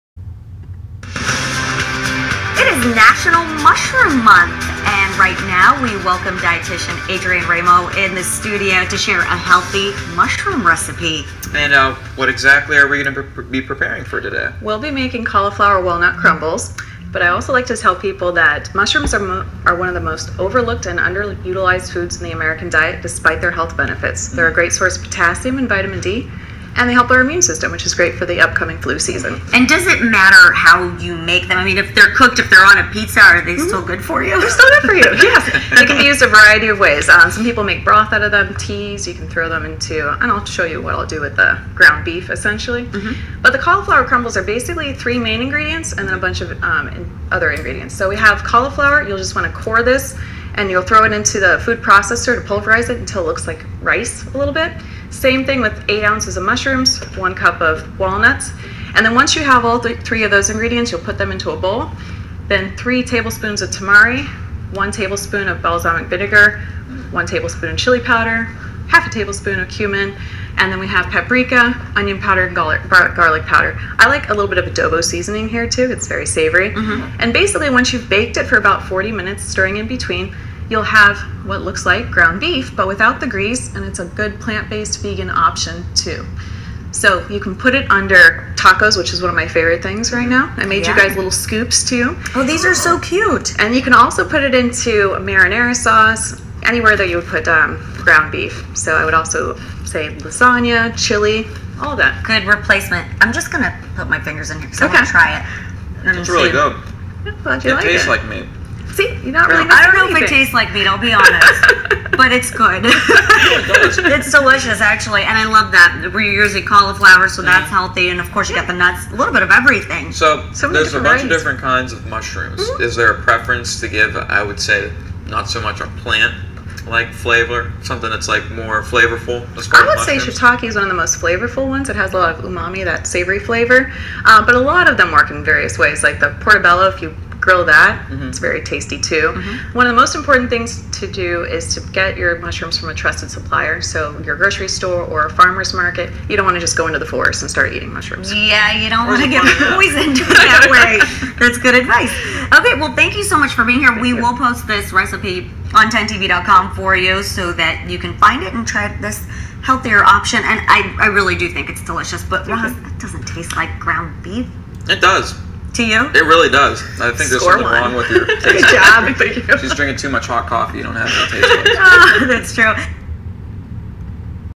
10TV: National Mushroom Month
Adrienne Raimo, RDN, L.D. of One Bite Wellness shares facts about mushrooms and how to use them in the popular Cauliflower & Walnut Crumbles recipe with Karina Nova and Ross Caruso of WBNS 10TV.